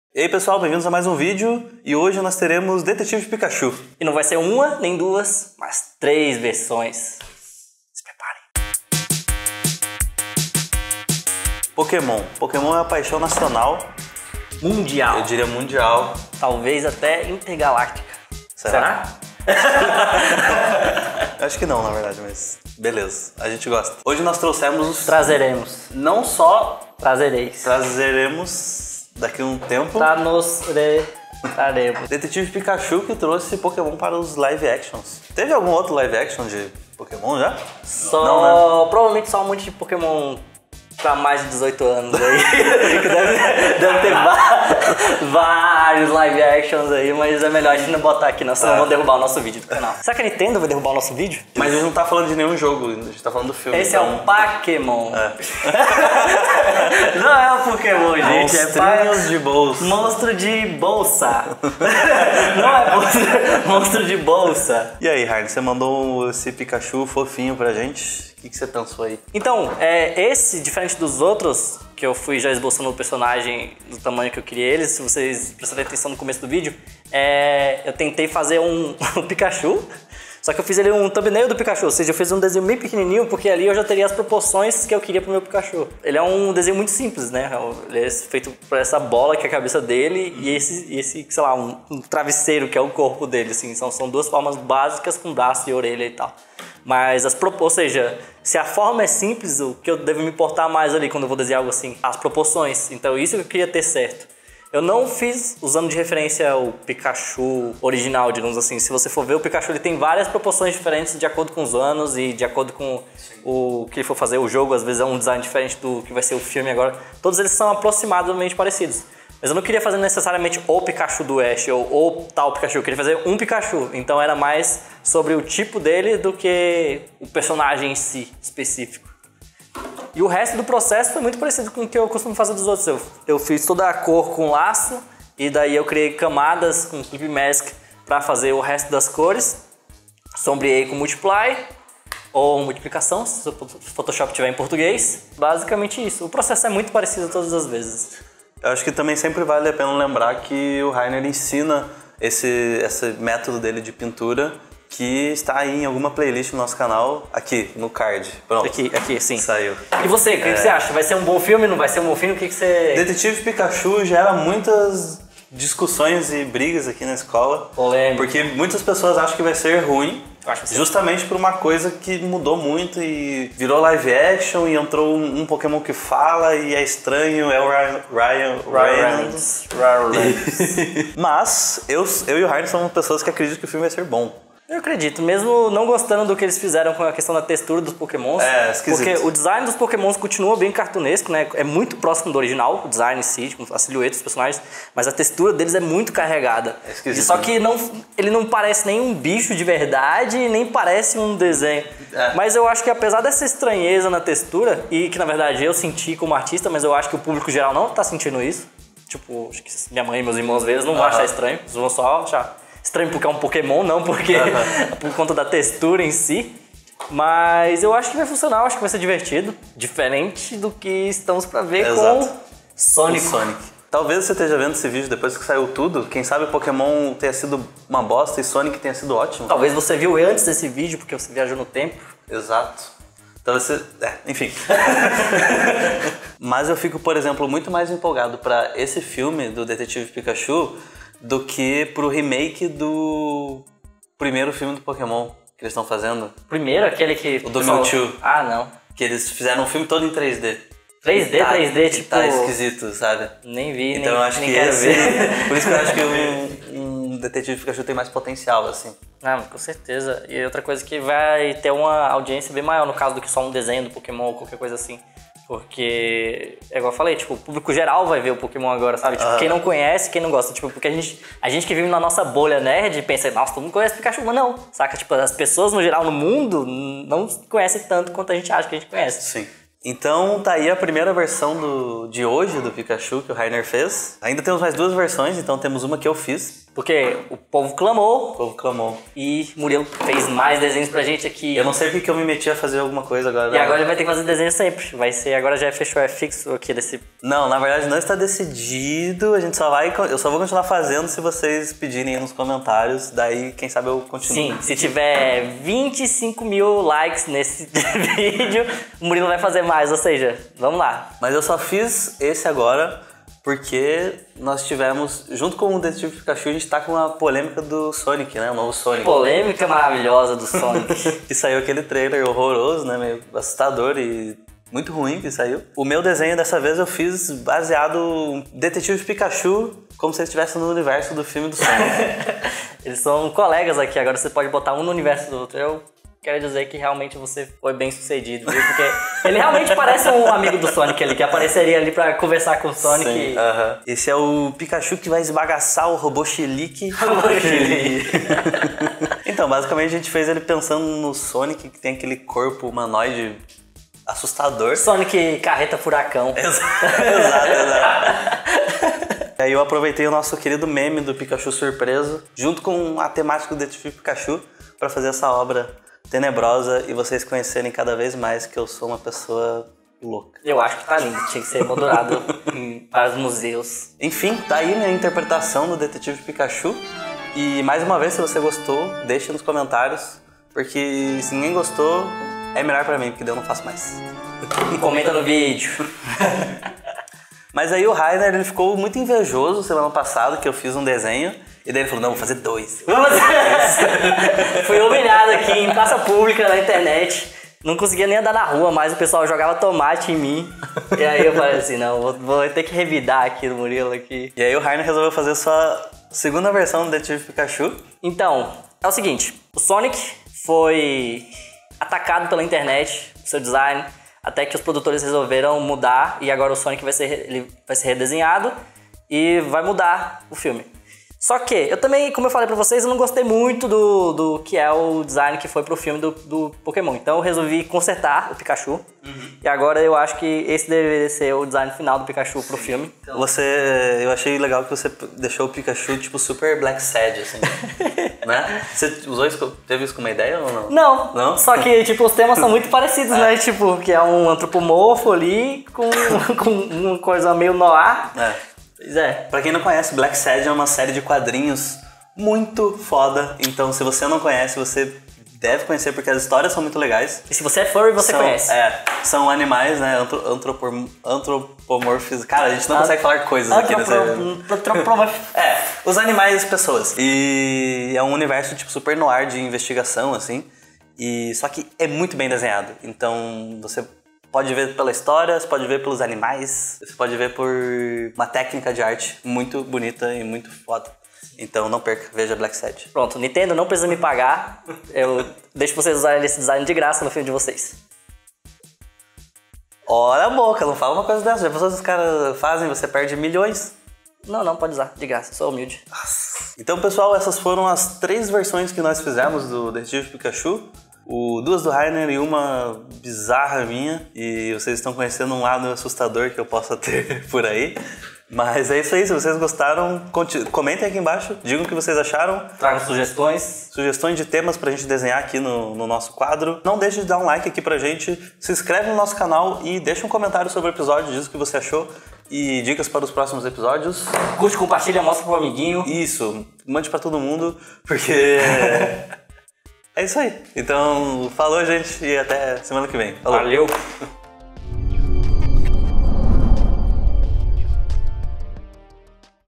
Desenhando Pokémon: Detetive Pikachu
Olá Pessoas! Nesse episódio do Workbox, Murilo Stupak e Rayner Alencar desenham e discutem sobre o live action de Pokémon: Detetive Pikachu!

:) Gostou do episódio? Deixe suas críticas e sugestões nos comentários!